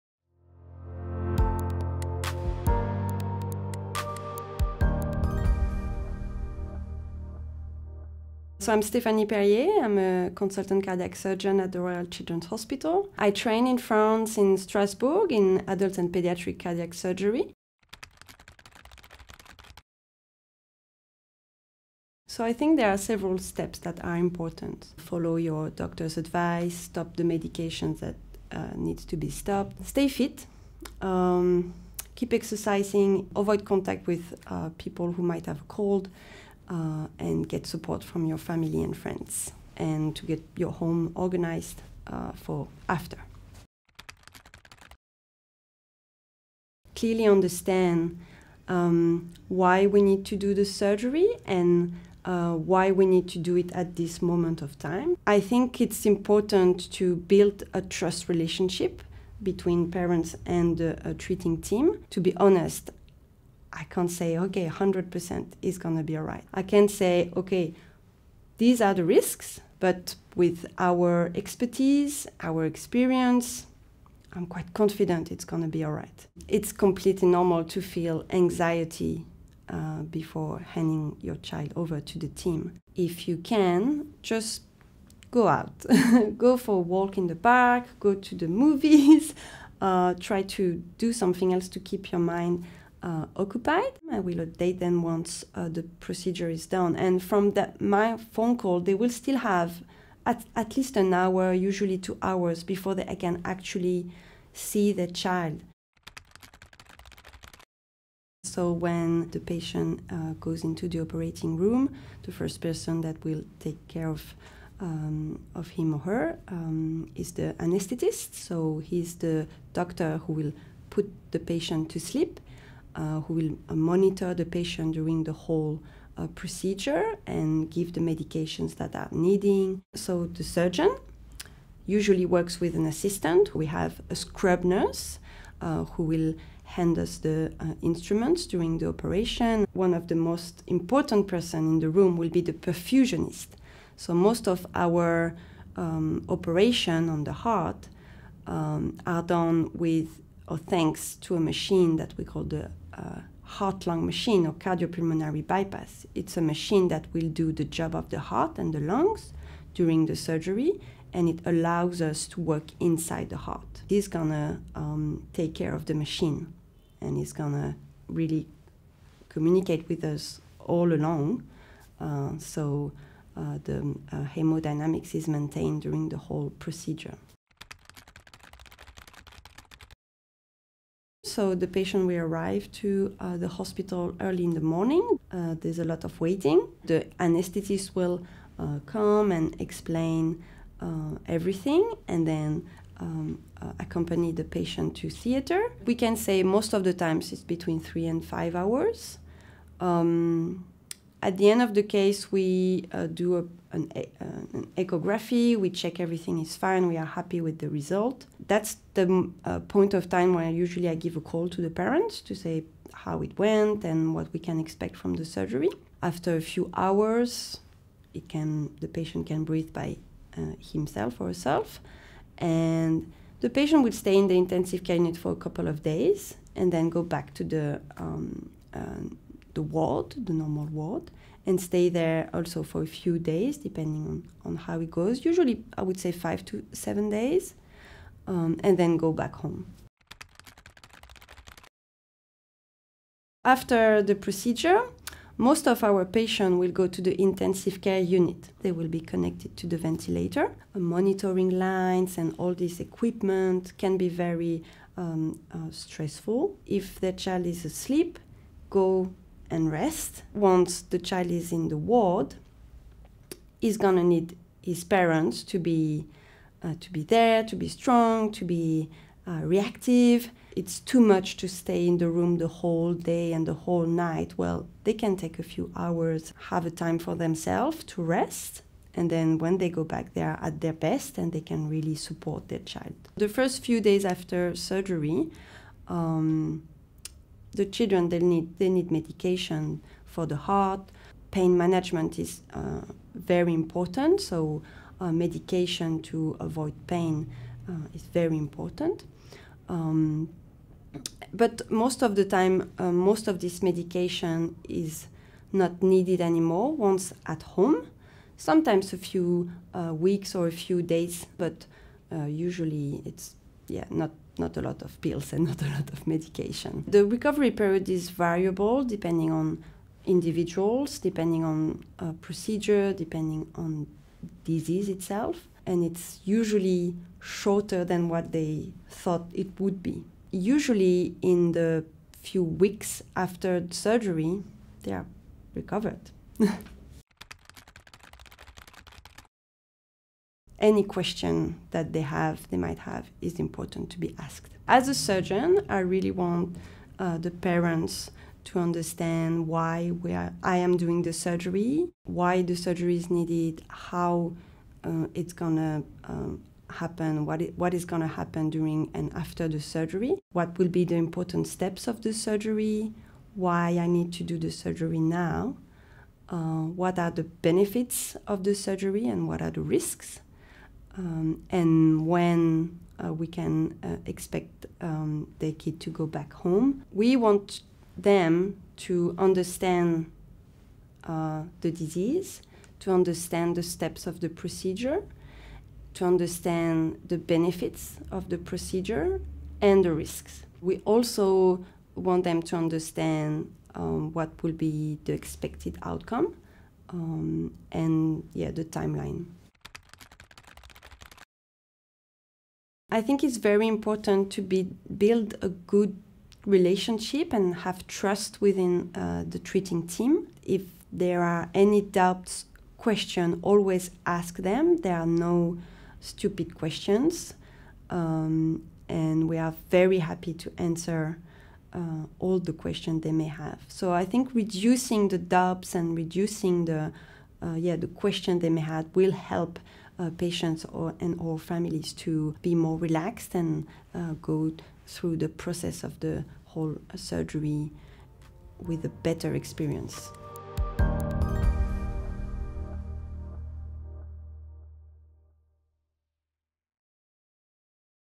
CoHD Explained: Open heart surgery
Join Dr. Stephanie Perrier, Consultant Cardiac Surgeon at the Royal Children's Hospital Melbourne, as she provides an overview of what to expect with open heart surgery for individuals and families impacted by Childhood-onset Heart Disease.

Gain insights into preparation, who's in the room during surgery, timing expectations, and the recovery process. Stephanie also provides helpful advice on questions to ask your surgeon and health team both before and after surgery.

Produced by HeartKids, this informative video provides practical guidance for individuals facing open-heart surgery as part of their CoHD journey.